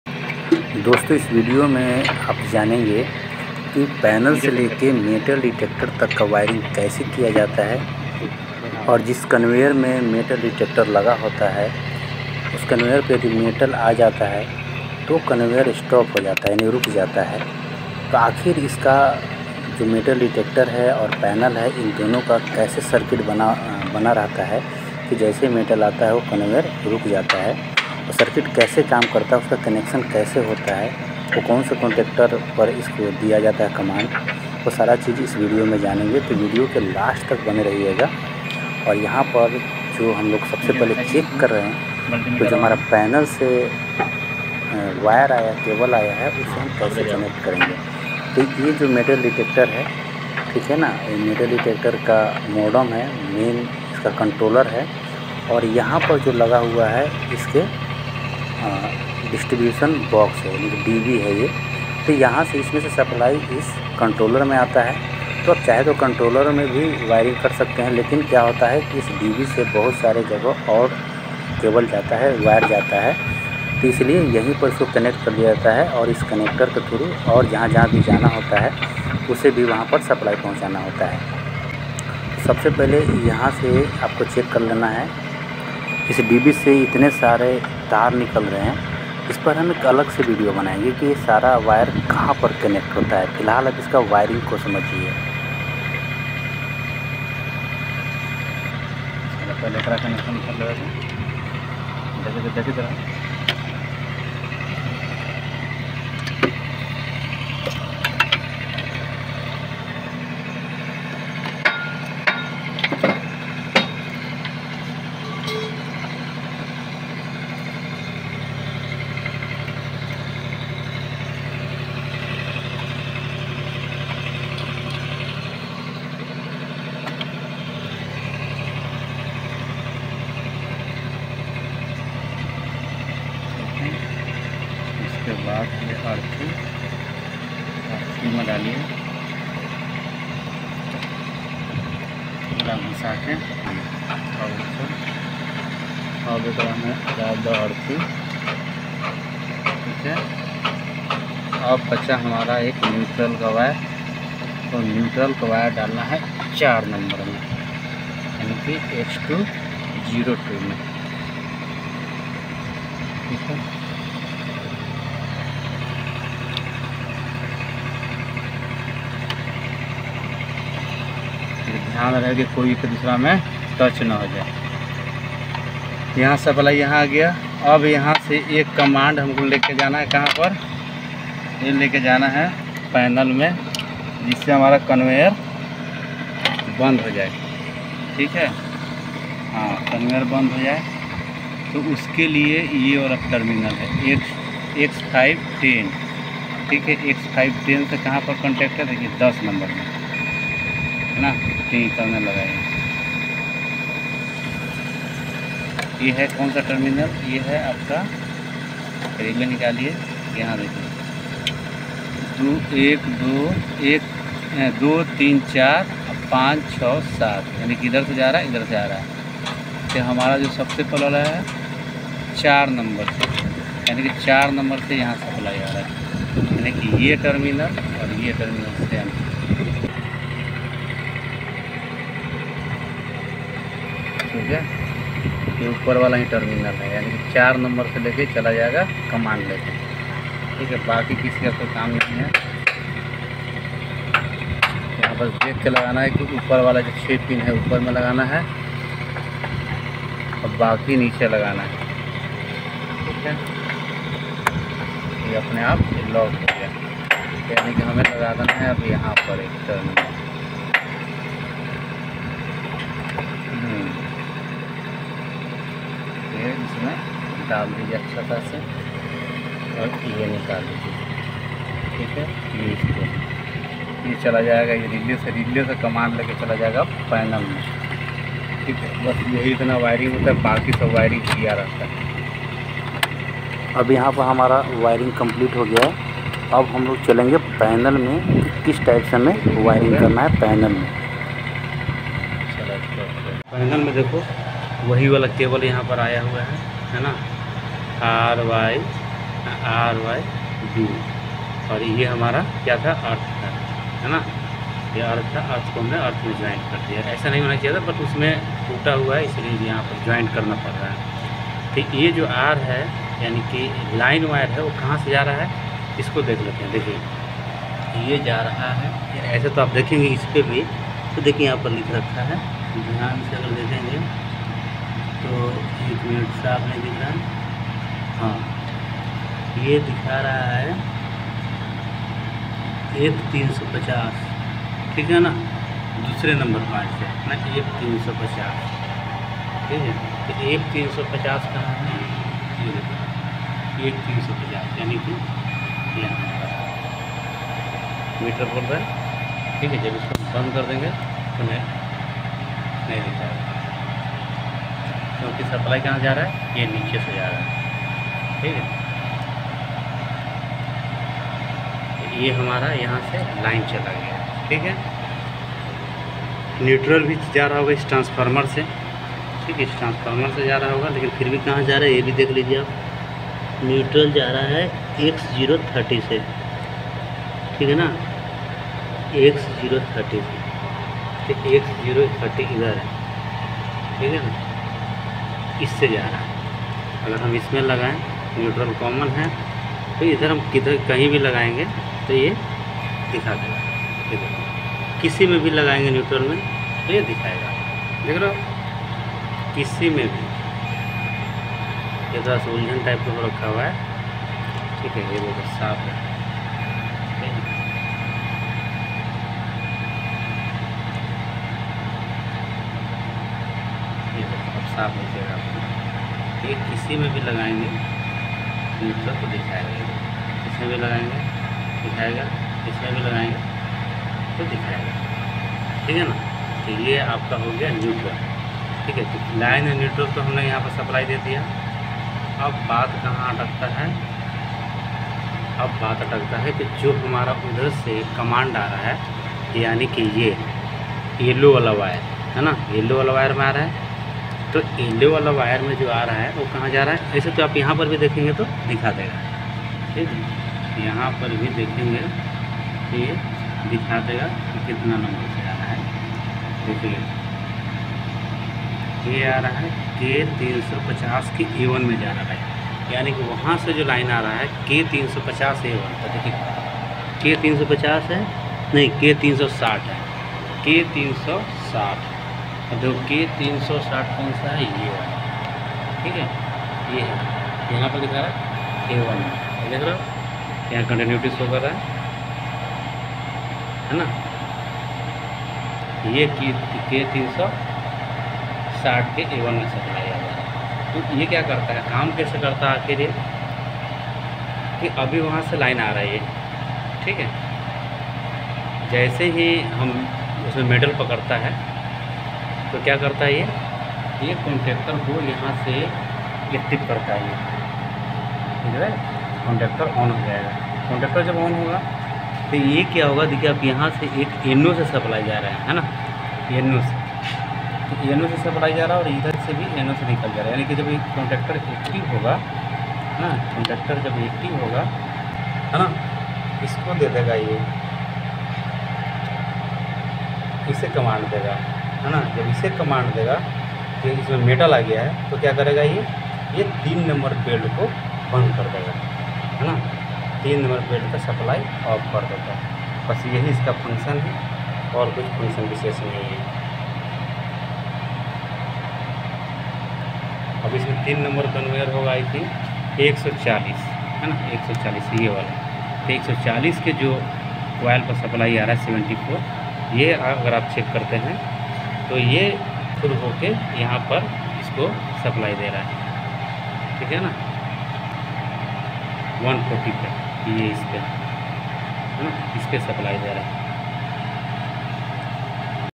Conveyor metal detector wiring | Metal detector Connection
Jai Hind,

Dosto ies video me aap complete janege ki belt conveyor metal detector ka wiring kaise karte hain , yani electrical panel se conveyor belt tak jis jagah metal detector laga hai waha tak ies video ko dekh kar aap aasani se wiring kar sakte hai.
Thanks